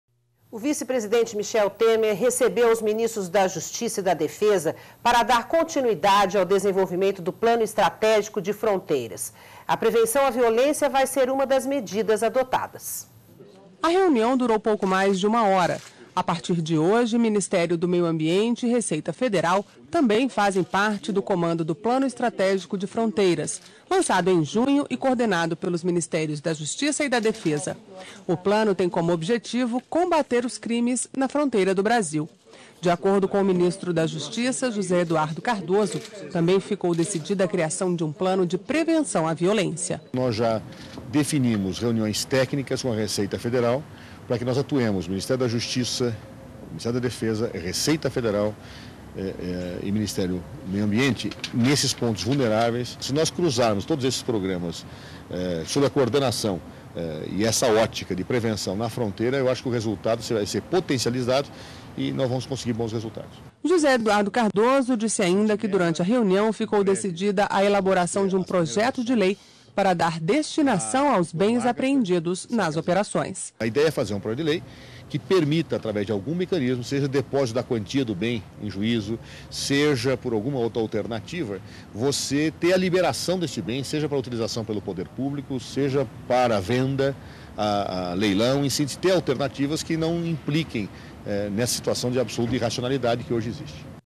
Plano Estratégico de Fronteiras também fará prevenção à violência
NBR NOTÍCIAS - 13.07.11: O vice-presidente Michel Temer recebeu os Ministros da Justiça e da Defesa para dar continuidade ao desenvolvimento do Plano Estratégico de Fronteiras. A prevenção à violência vai ser uma das medidas adotadas.